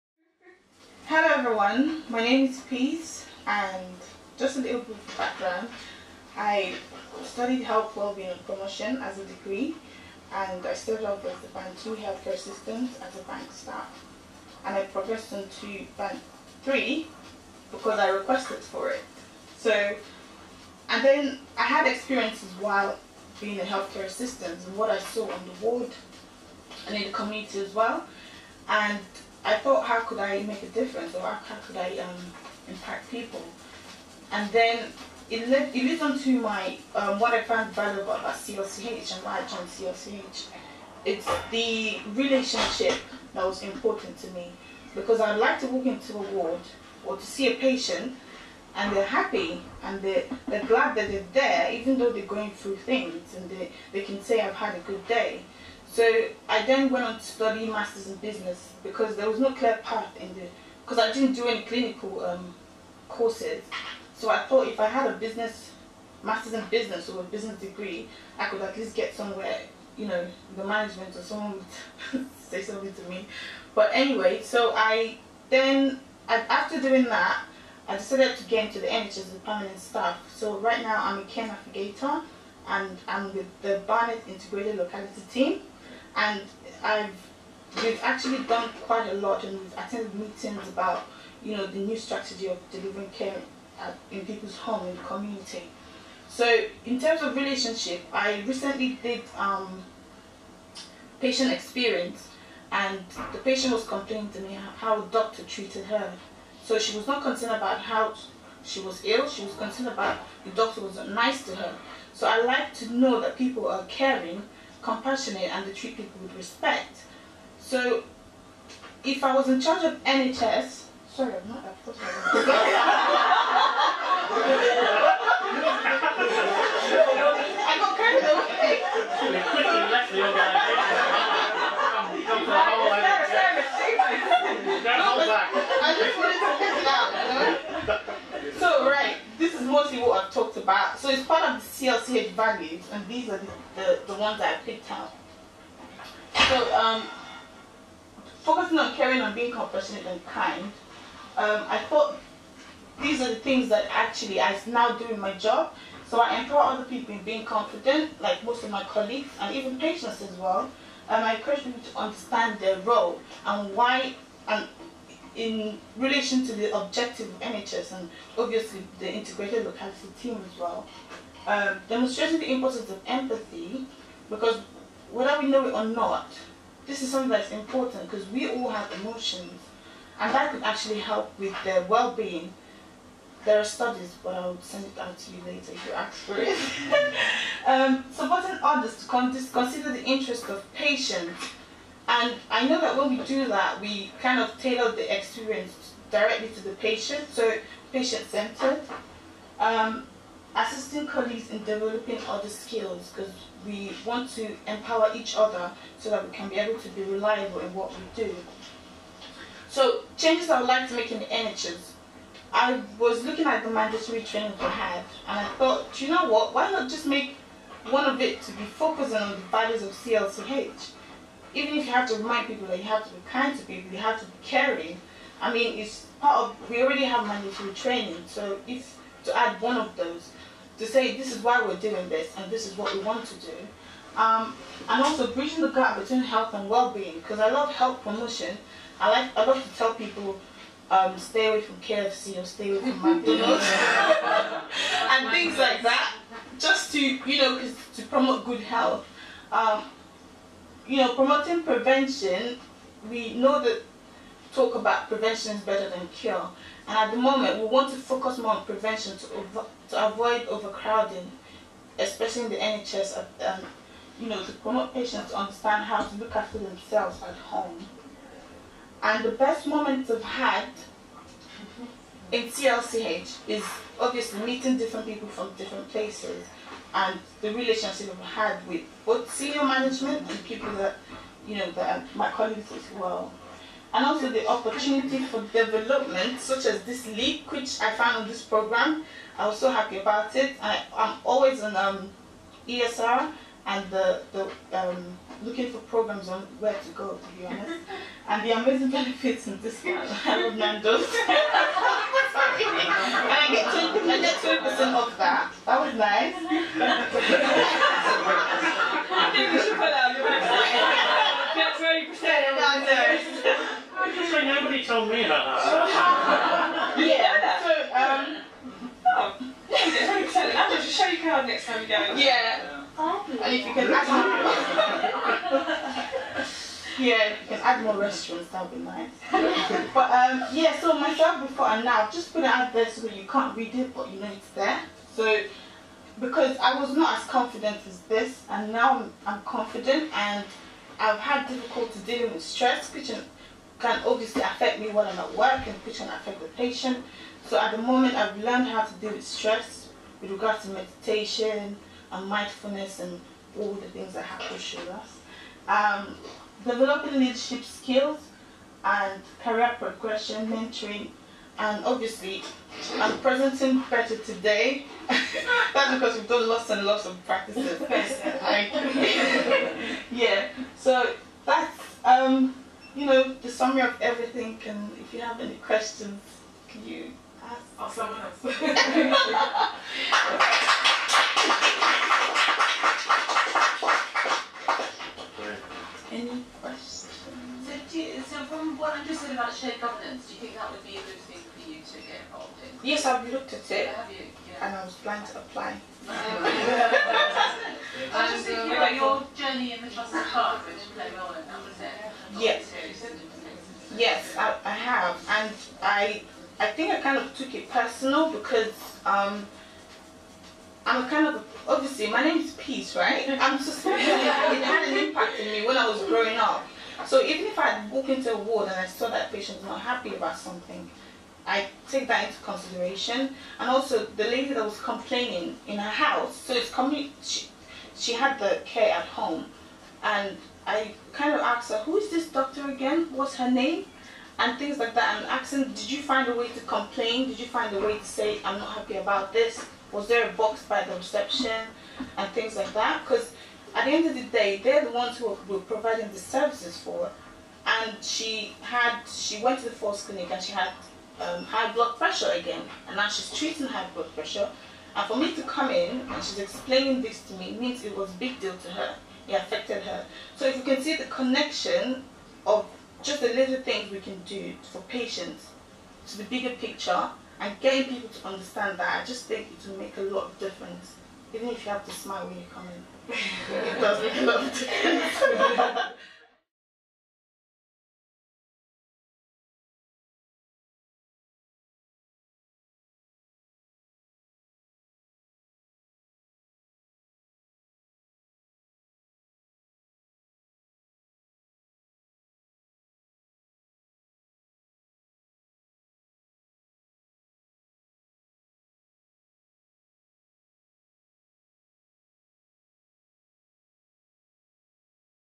LEAP presentation with Peace Ossai - Care Navigator
Giving a presentation, to senior executives on the final day of the NHS CLCH LEAP programme.